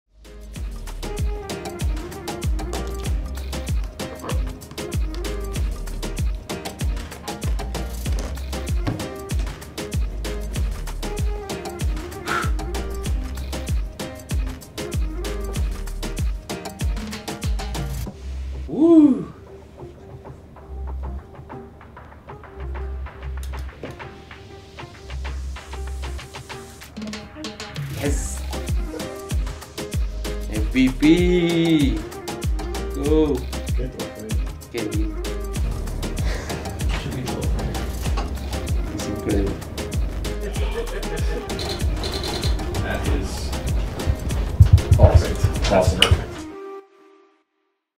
Luciano Acosta unboxes his 2023 MLS MVP award
In addition to helping FC Cincinnati win the Supporters’ Shield, Lucho Acosta’s MVP season powered the club to its second consecutive Audi MLS Cup Playoffs berth and helped FCC qualify for the Concacaf Champions Cup for the first time in club history.